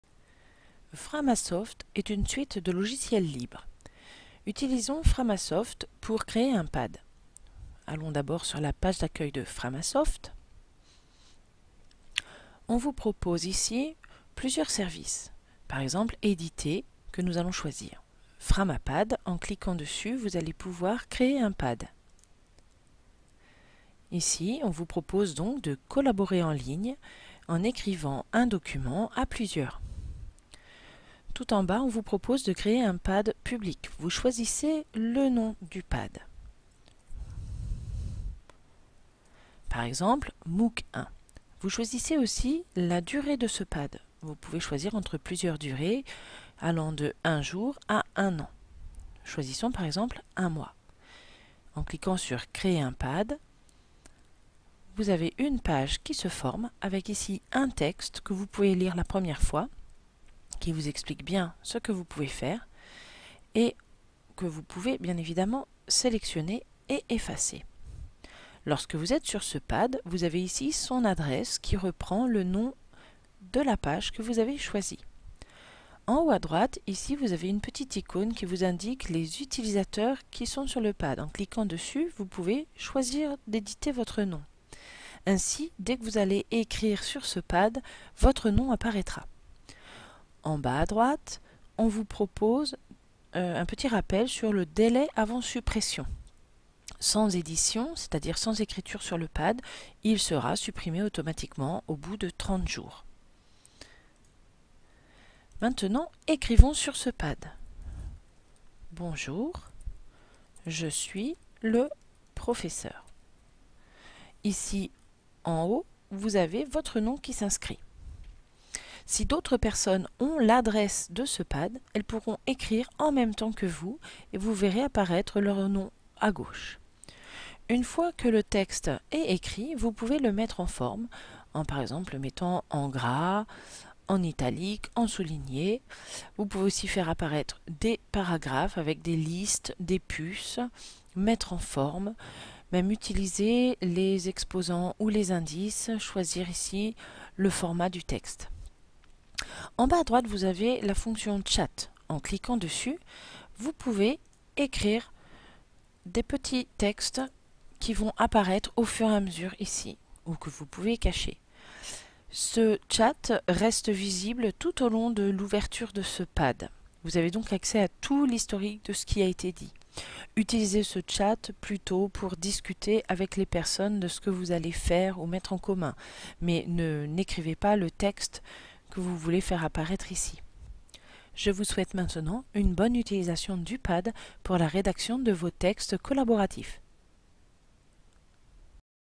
Tutoriel Framapad
Framasoft est un site de logiciels, qui propose des outils libres et gratuits pour collaborer et travaille.
Présentation de l'outil "Framapad" qui est un logiciel de traitement de texte en ligne, utilisable par plusieurs personnes en même temps, et qui reste accessible pendant une durée à définir.